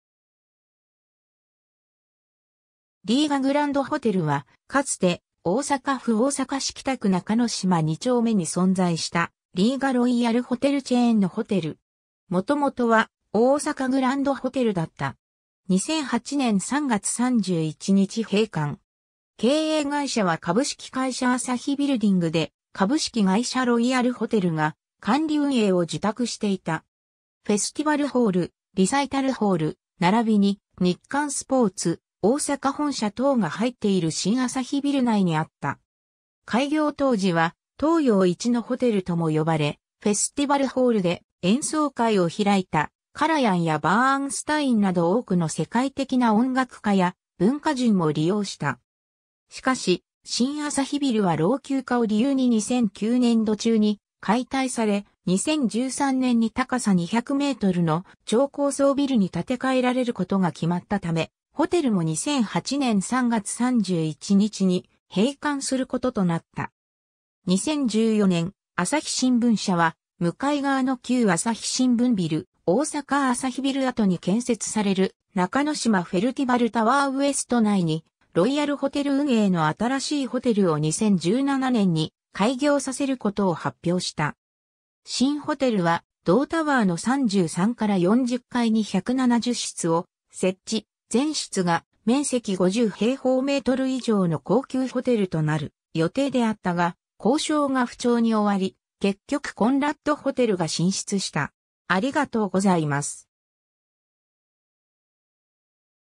リーガグランドホテル
リーガグランドホテルは、かつて大阪府大阪市北区中之島二丁目に存在したリーガロイヤルホテルチェーンのホテル。
元々は大阪グランドホテルだった。
2008年3月31日閉館。
 経営会社は株式会社朝日ビルディングで、株式会社ロイヤルホテルが管理運営を受託していた。
 フェスティバルホール、リサイタルホール、並びに日刊スポーツ大阪本社等が入っている新朝日ビル内にあった。
開業当時は東洋一のホテルとも呼ばれ、フェスティバルホールで演奏会を開いたカラヤンやバーンスタインなど多くの世界的な音楽家や文化人も利用した。
 しかし、新朝日ビルは老朽化を理由に2009年度中に解体され、2013年に高さ200mの超高層ビル（中之島フェスティバルタワー）に建替えられることが決まったため、ホテルも2008年3月31日に閉館することとなった。
 2014年、朝日新聞社は向かい側の旧朝日新聞ビル・大阪朝日ビル跡に建設される「中之島フェルティバルタワーウエスト」内に、ロイヤルホテル運営の新しいホテルを2017年に開業させることを発表した。
新ホテルは同タワーの33〜40階に170室を設置、全室が面積50平方メートル以上の高級ホテルとなる予定であったが、交渉が不調に終わり、結局コンラッド・ホテルが進出した。